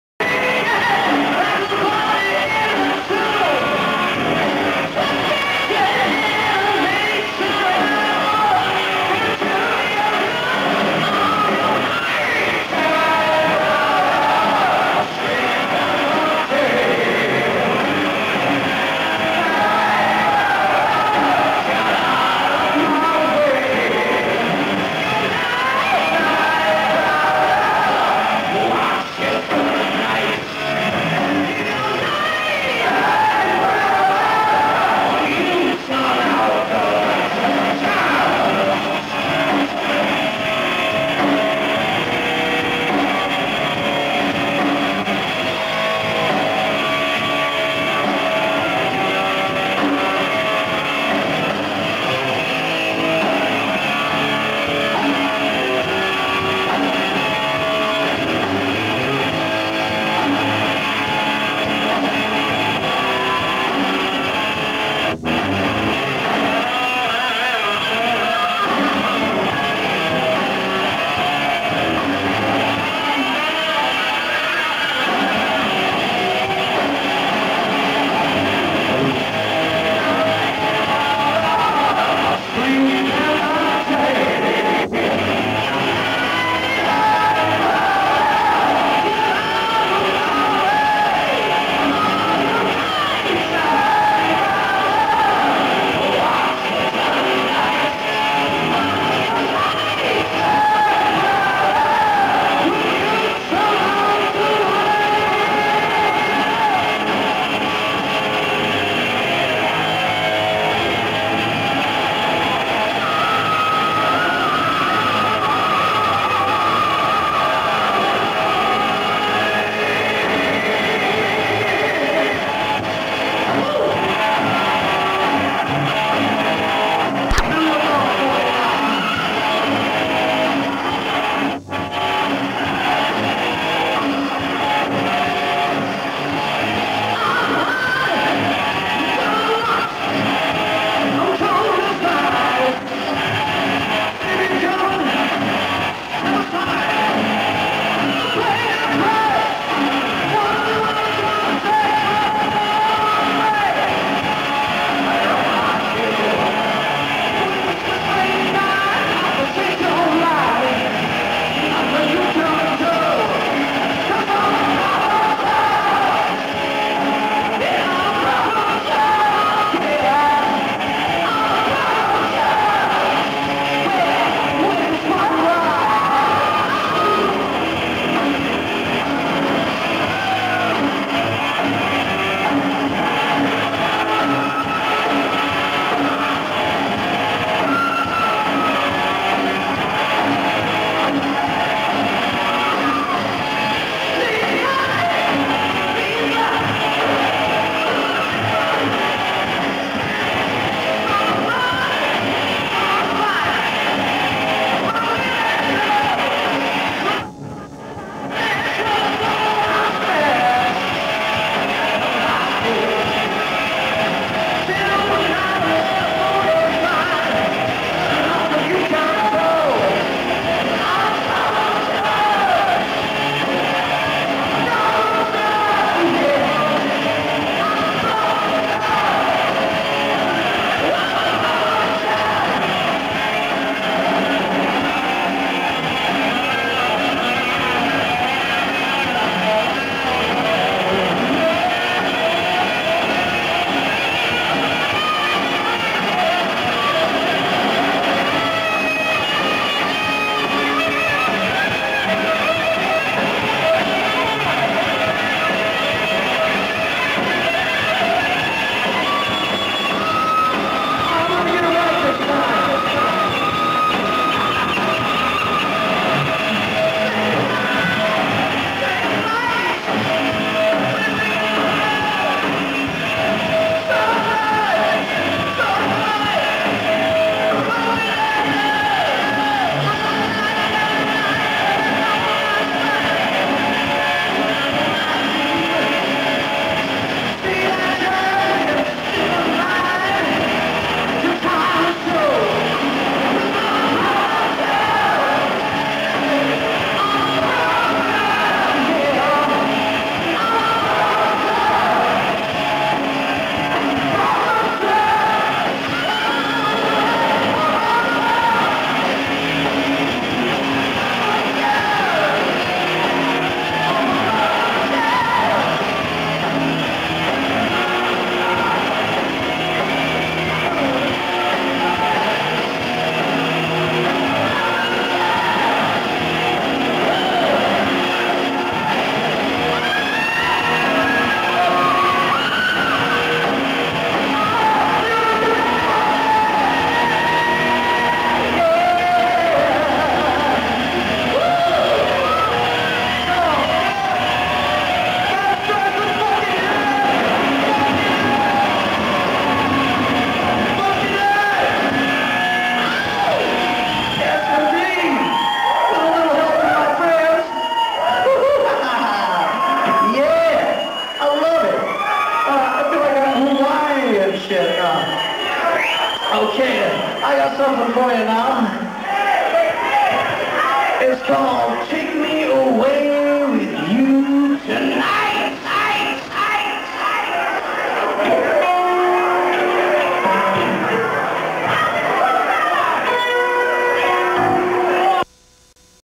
Maxx Thrust Official old Saratoga Winners Vid
Old video, but incredible fun! Watch other players jump on stage and join the fray!! Some created by other sources and obtained permission to display here. If you deserve credit for this video please let us know and we'll link you ASAP!